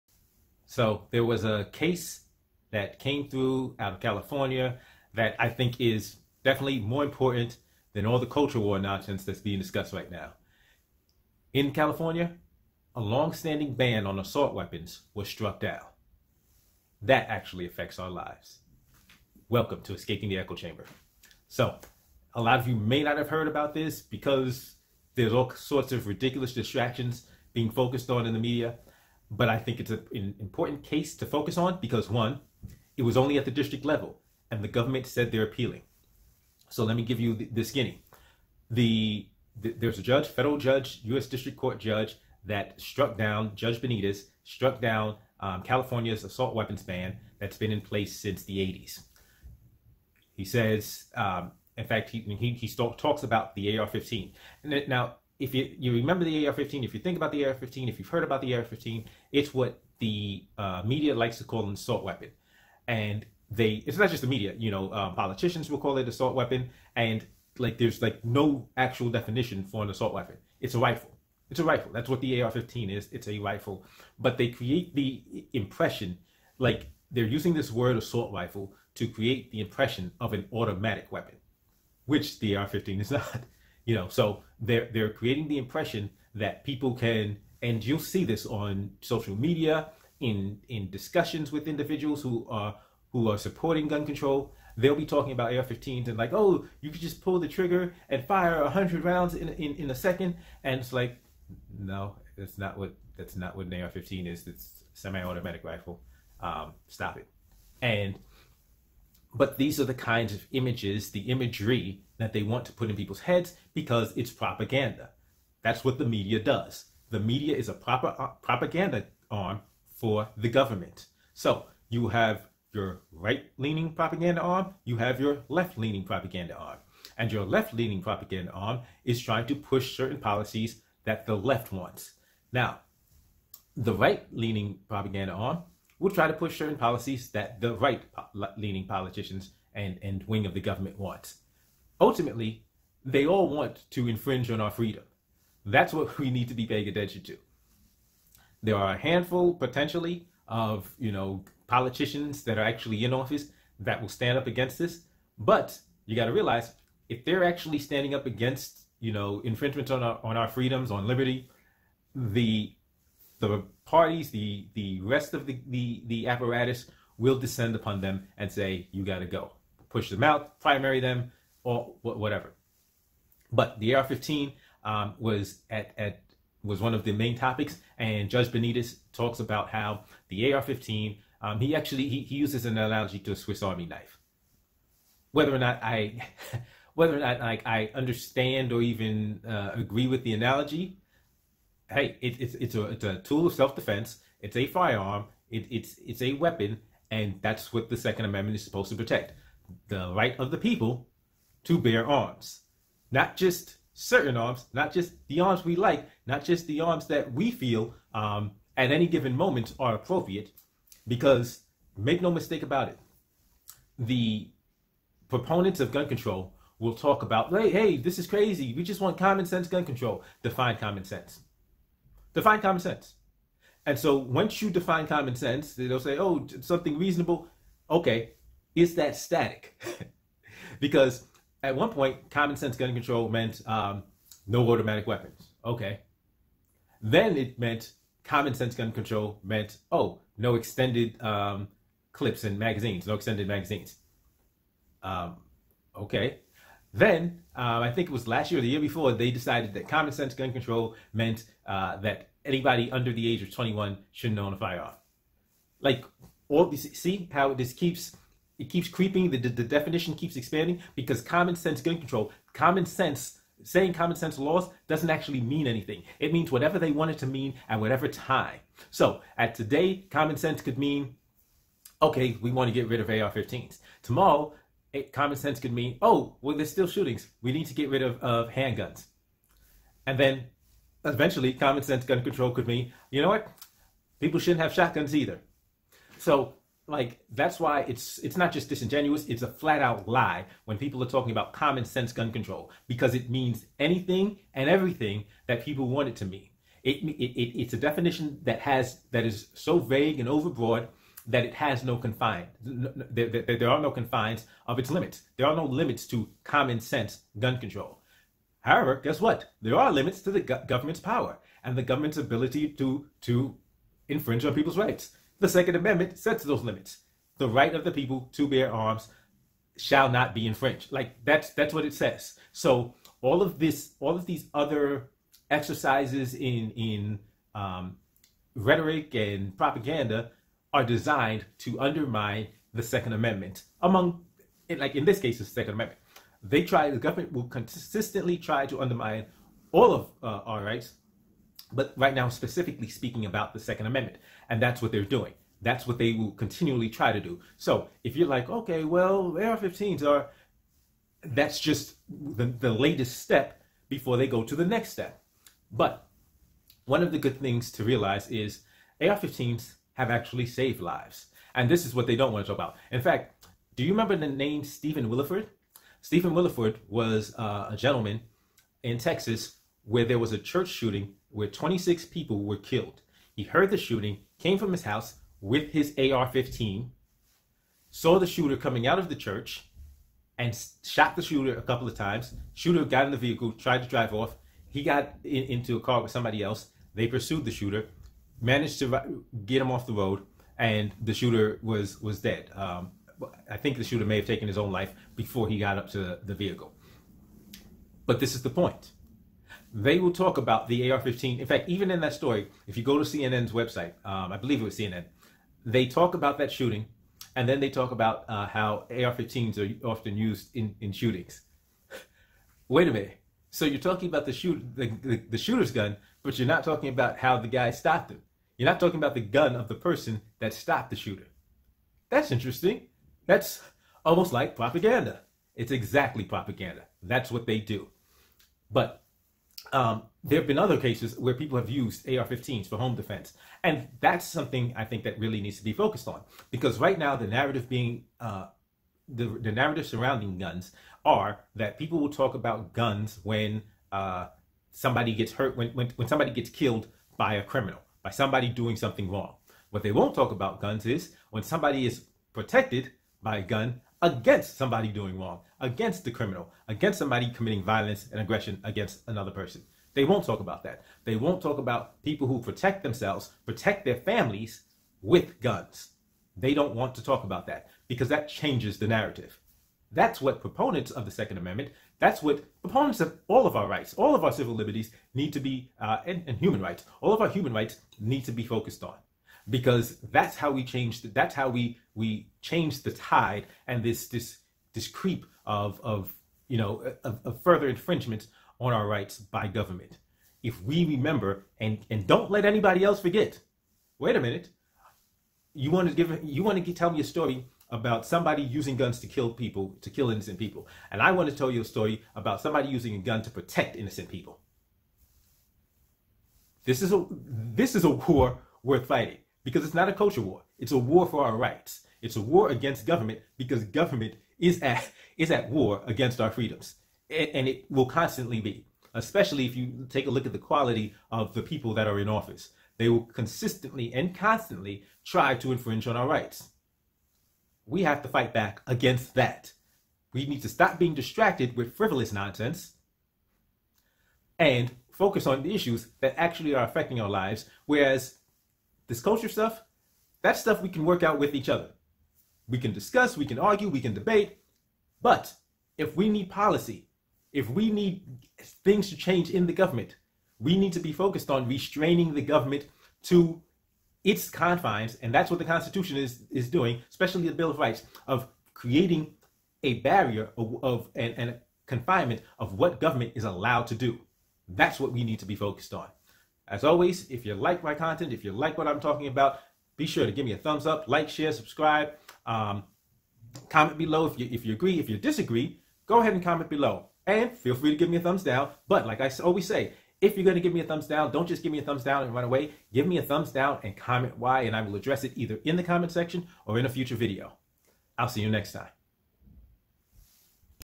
Is Owning An AR 15 Common Sense ?
AR 15 or common sense gun control? A Federal Judge doesn't think that's the choice and shot down an "assault weapons" ban. He ruled that the AR 15 is a great weapon for home defense. "Common Sense" gun control advocates in the government will appeal but I discuss why the phrase common sense is just a smoke screen for infringement.